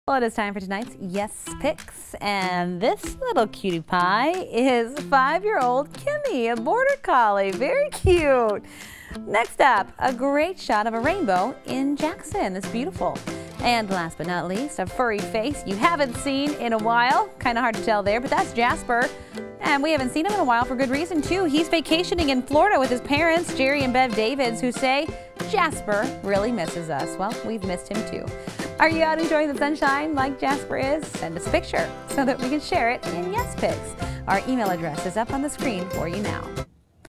YES! Pics: 2/20/17
Check out these photos submitted by viewers that made it on the air on Monday, 2/20/17.